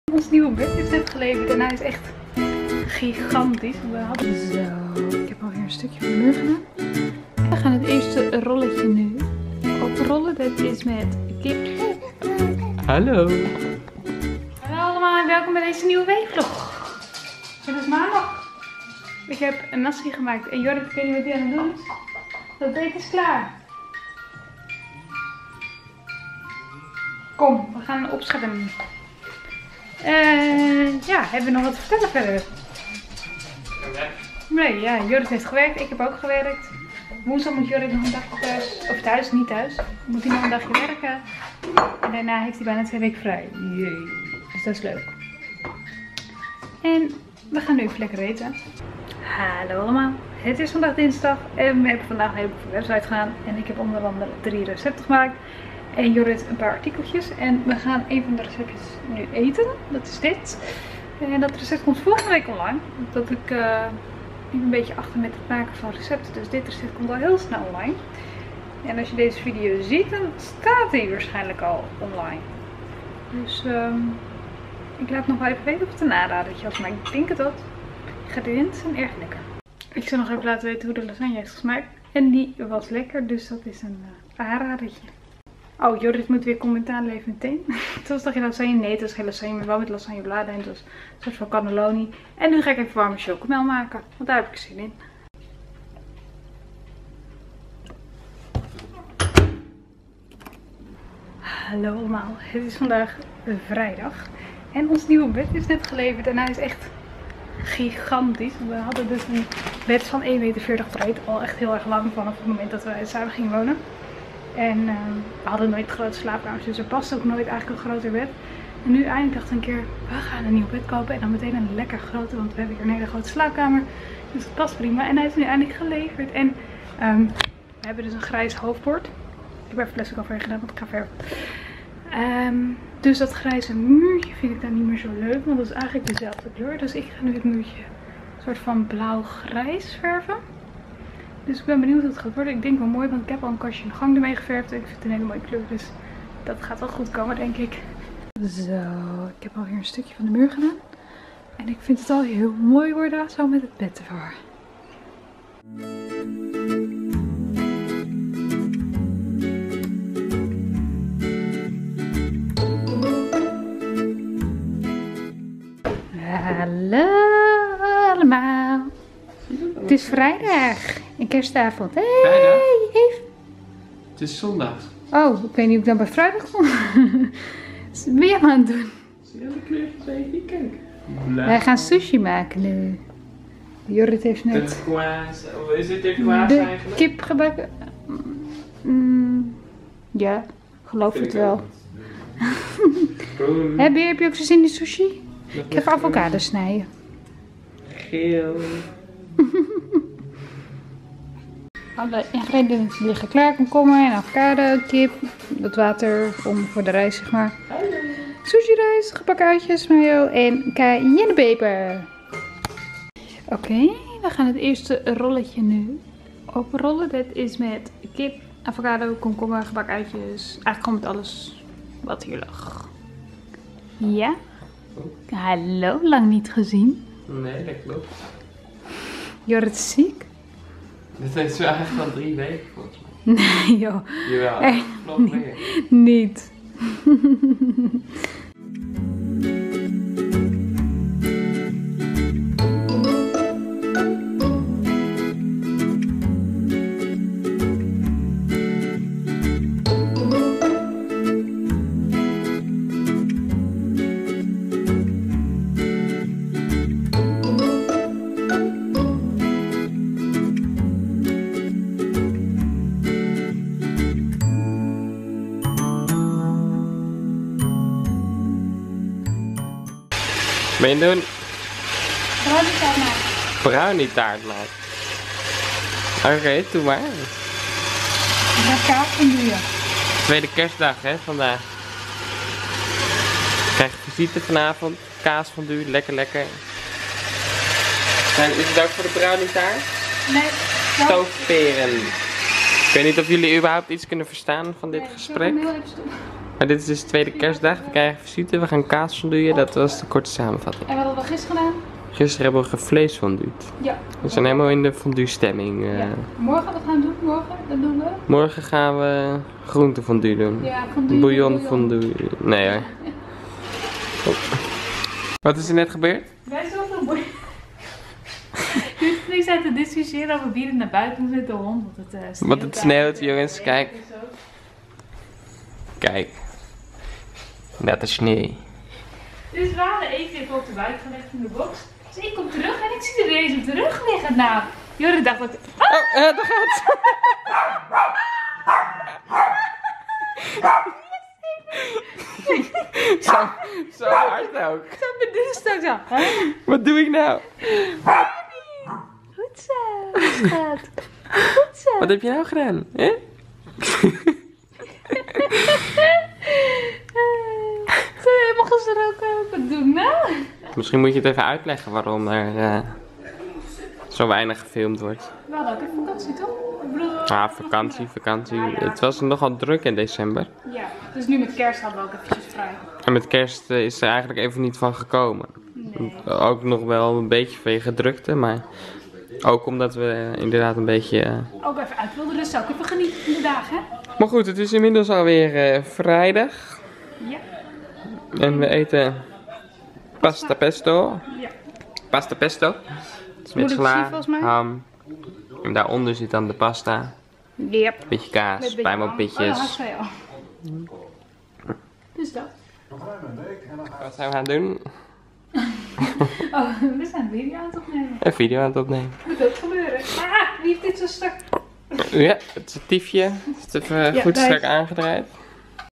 NIEUW BED & MUUR VERVEN VLOG #116 | LEKKER EN SIMPEL
We geven jullie weer een klein kijkje in ons leven!